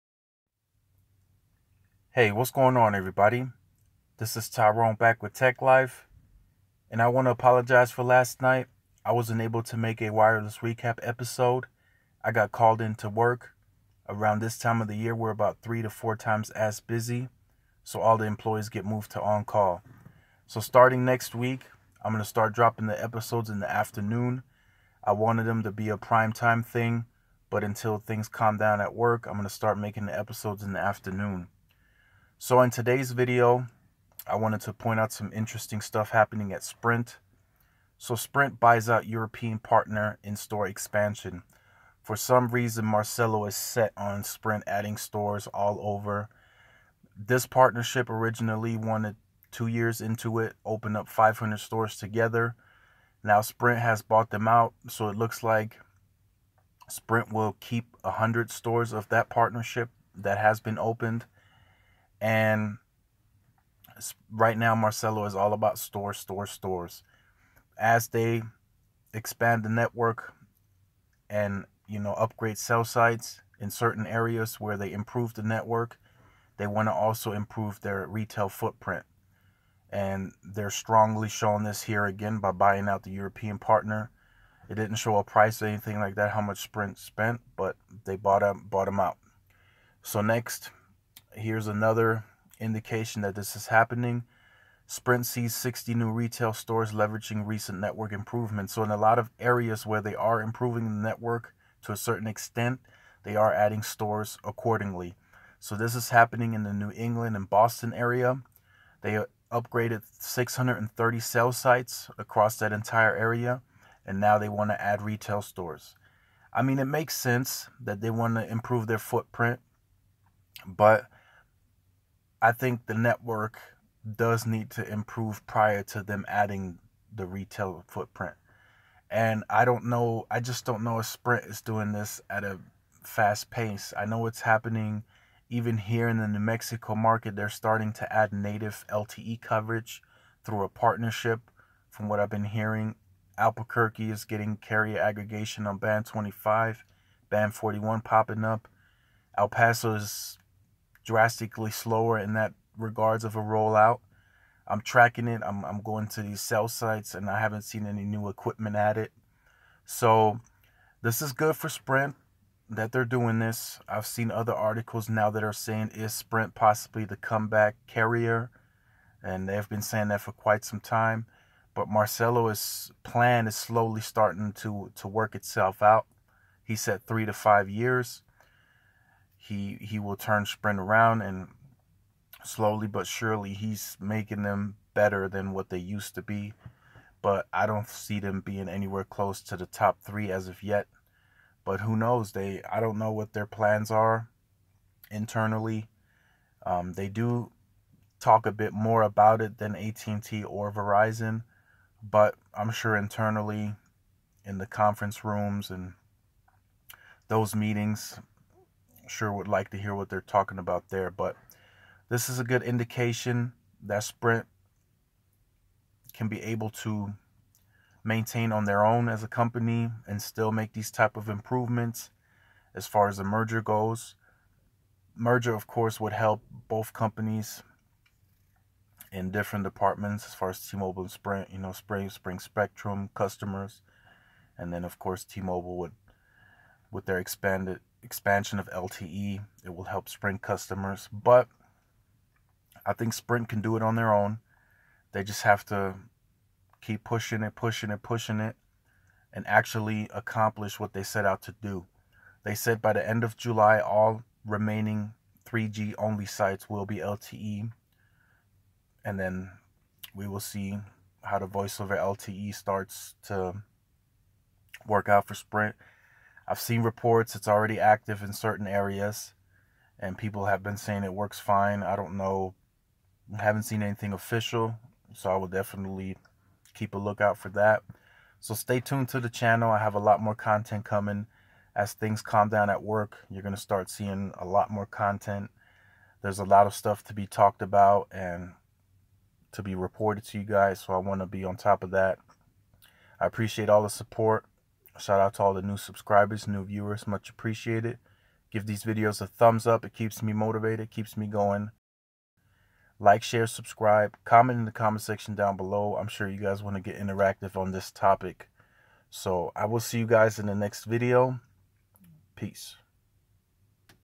SPRINT WIRELESS | SOME INTERESTING STUFF HAPPENING AT SPRINT
In today's video I wanted to report to you guys that sprint is becoming very aggressive in rolling out more retail locations.